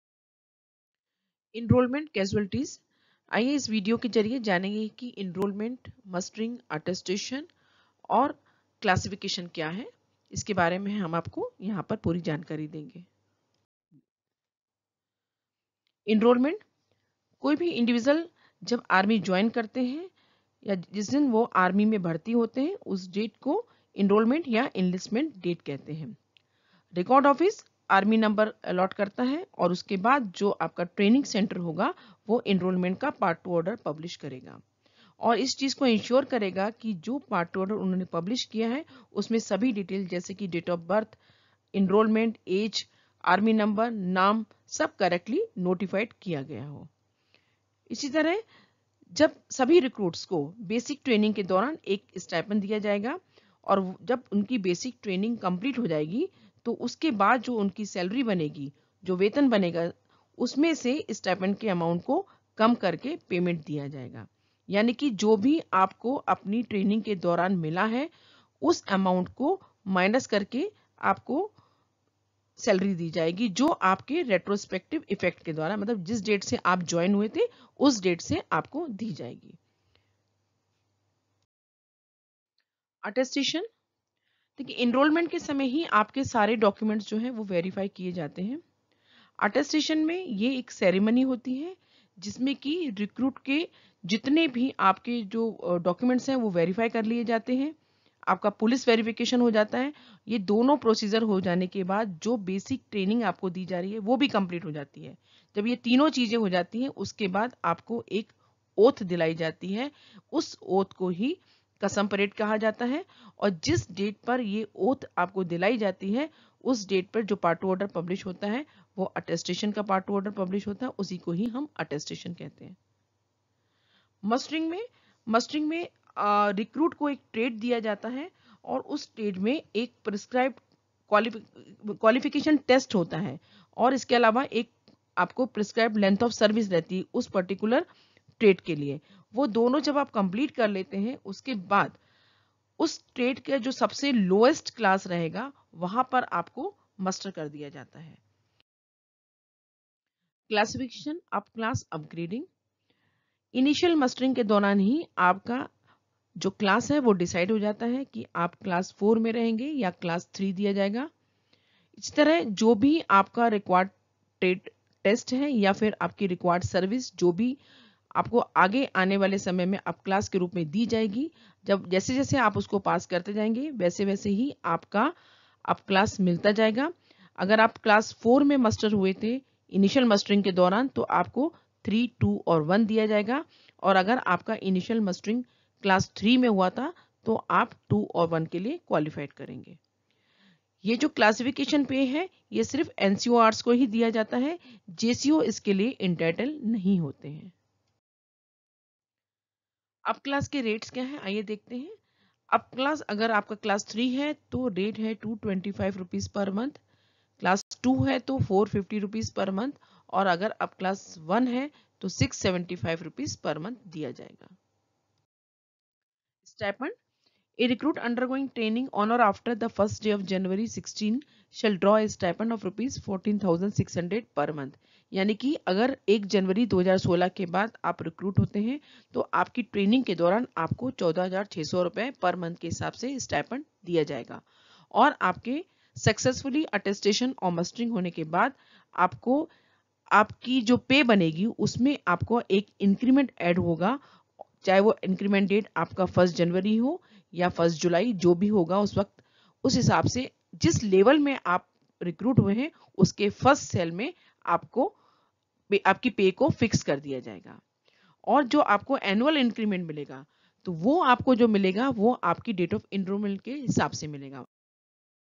Video 2.1 Enrolment casualities
CGDA Shri Rajnish Kumar, IDAS launched the KSHIPRA 2.O video series in May 2022 at PCDA Southern Command, Pune.

KSHIPRA 2.0 is a series of educational videos prepared by the office of the PCDA (SC) Pune to familiarize the Army Jawans about their Pay and entitlements from recruitment to their retirement.

A copy of the video lectures has also been shared by PCDA Southern Command to all record offices and PAOs.